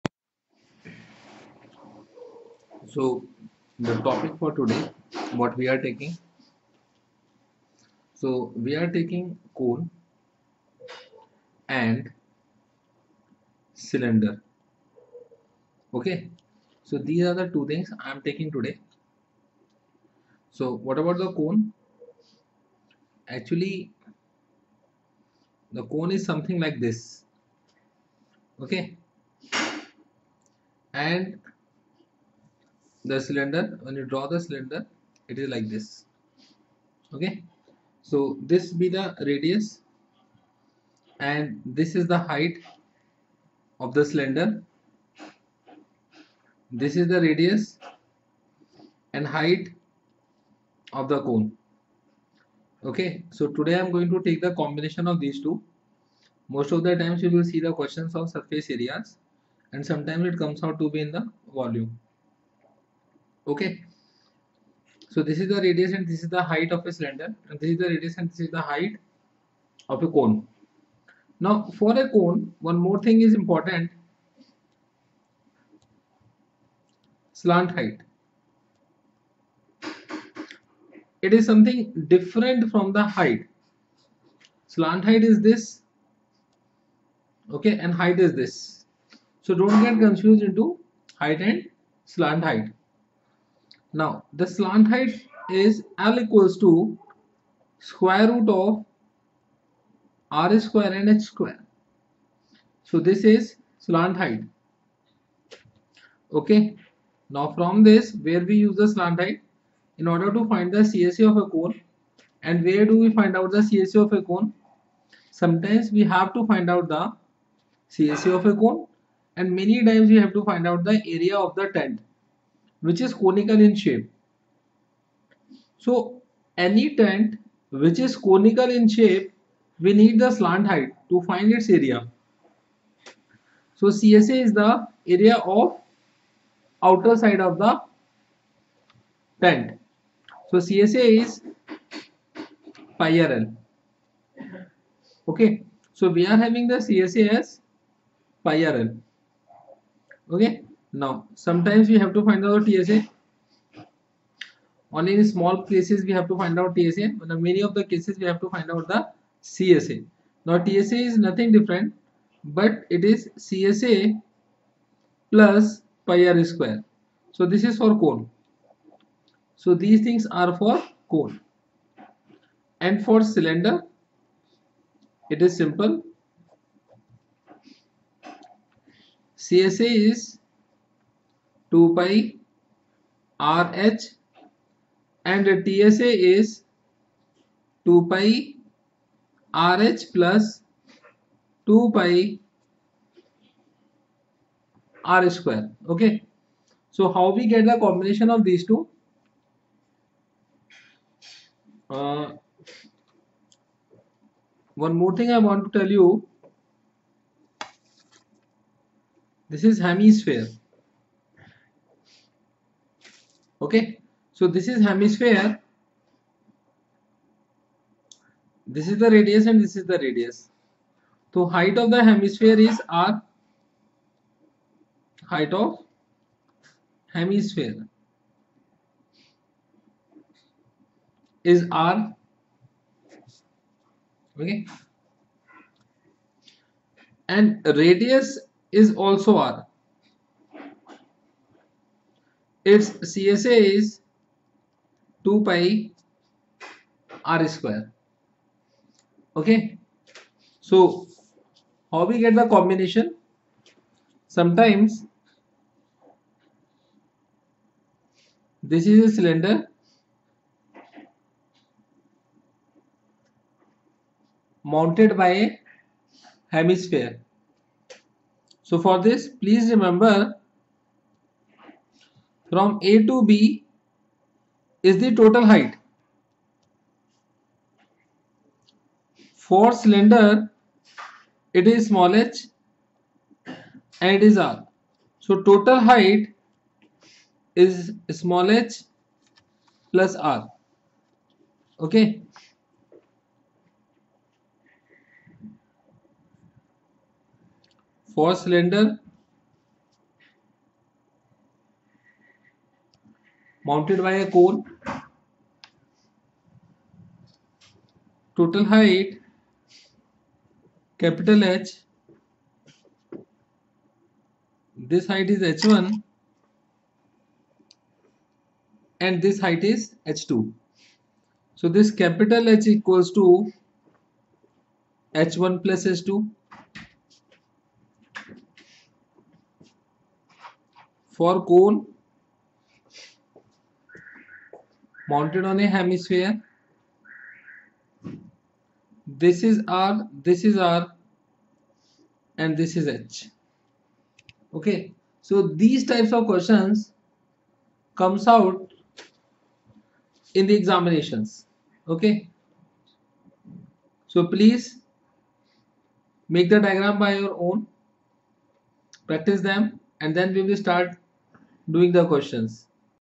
class 9th cone and cylinders I
A cone is a three-dimensional geometric shape that tapers smoothly from a flat base (frequently, though not necessarily, circular) to a point called the apex or vertex.